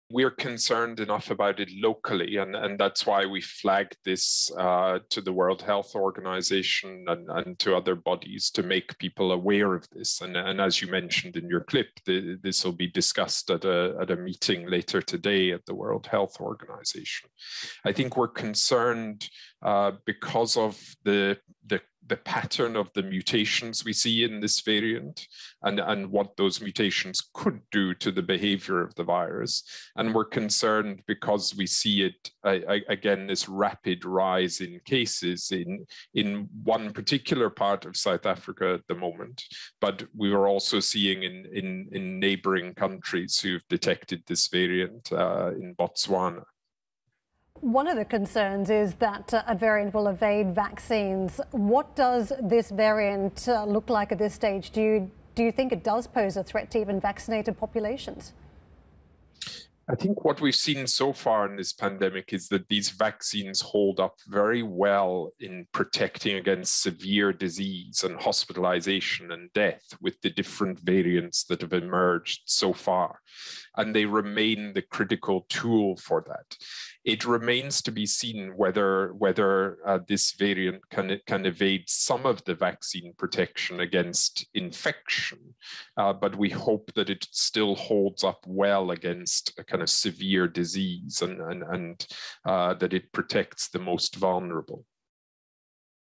Particular concern of the rapid rise of this new Covid variant from South Africa, expert says
Dr. Richard Lessells, an infectious disease physician at the University of KwaZulu-Natal, discusses the Covid-19 variant detected in South Africa.